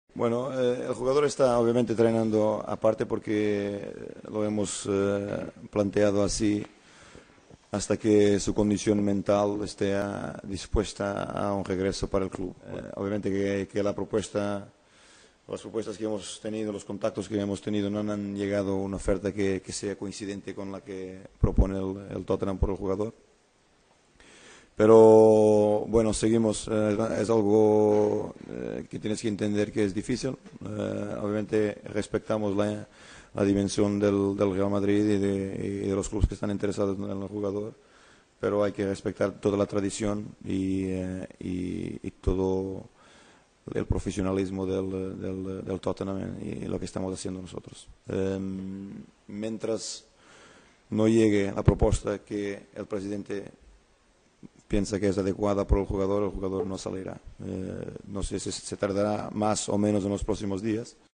Villas-Boas habla sobre Modric
André Villas-Boas, el entrenador del Tottenham, reconoció el jueves que la situación de Modric es complicada dentro de su equipo. En un tono que evidenciaba cierto resentimiento por la pérdida del jugador, el técnico luso aseguraba que "mientras no llegue una propuesta adecuada para el presidente por el jugador, el jugador no saldrá". A lo que añadía que será cuestión de más o menos tiempo pero que en los próximos días se resolverá, en un sentido o en otro.